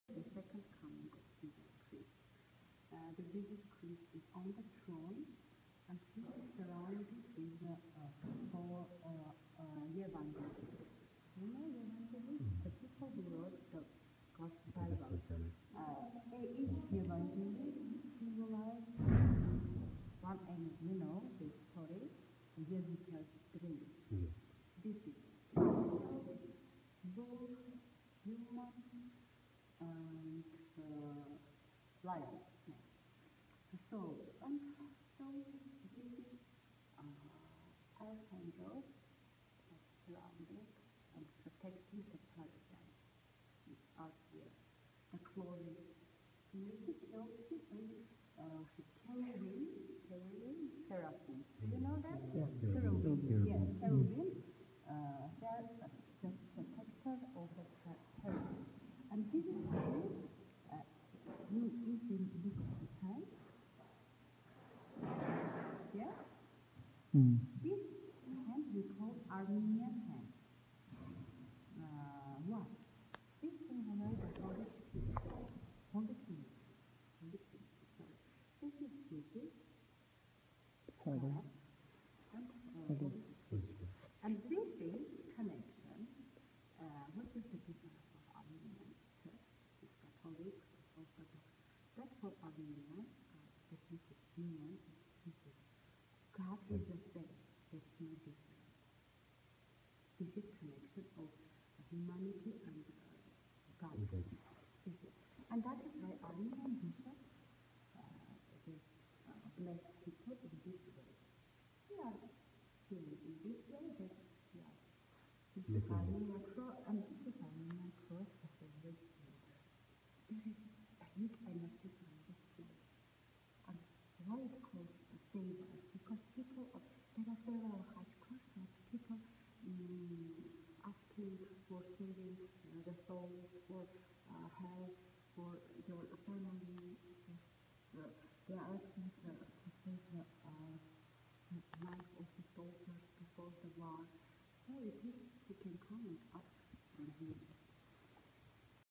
Armenia Sevanavank Monastery.
Sevanavank is a monastic complex located on a peninsula at the northwestern shore of Lake Sevan in the province of Armenia.